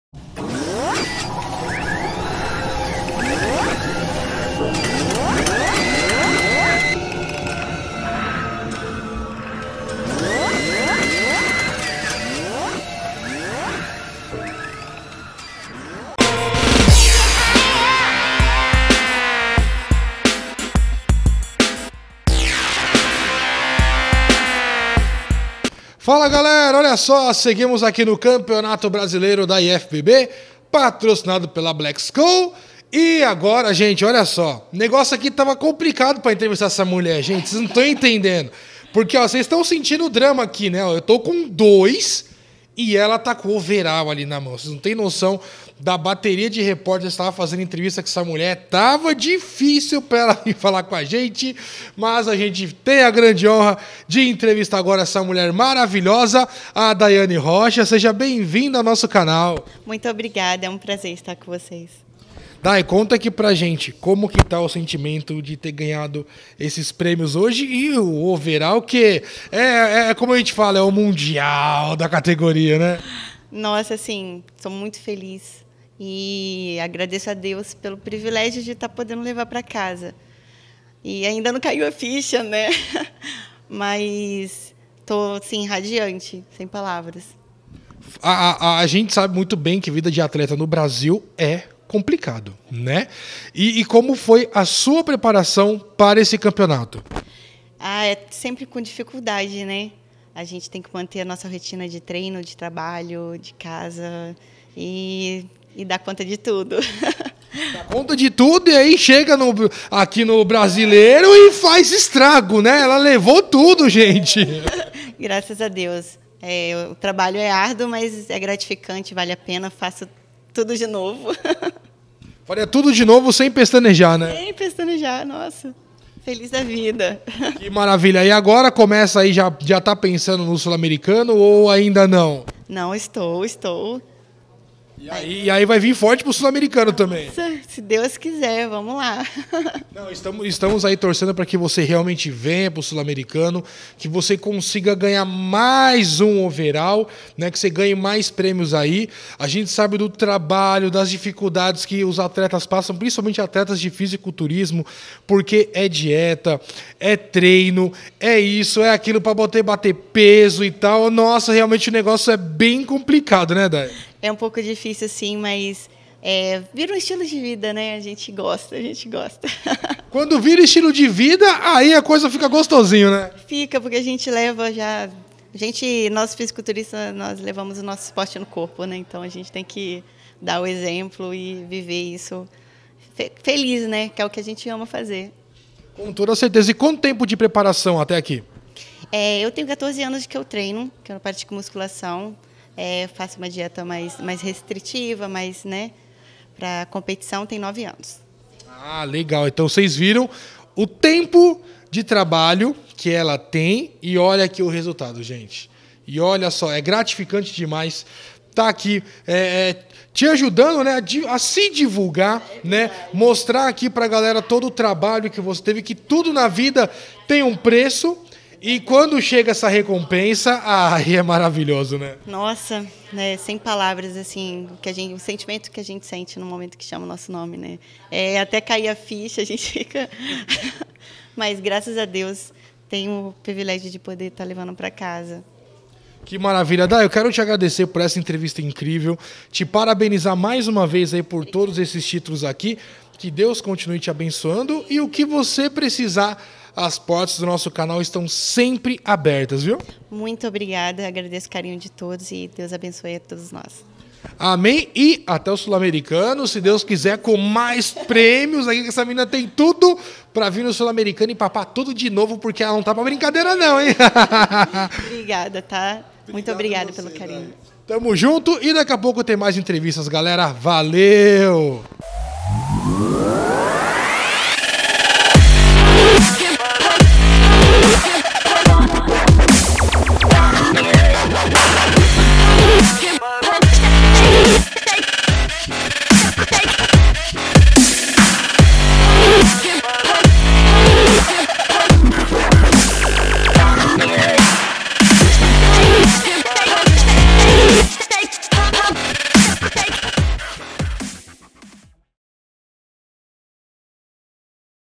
Os Papa Famoso "Campeonato Brasileiro - IFBB 2021" - Entrevista com Daiane Rocha
Neste vídeo, vocês acompanham tudo que rolou no Campeonato Brasileiro da IFBB 2021 e entrevistas exclusivas.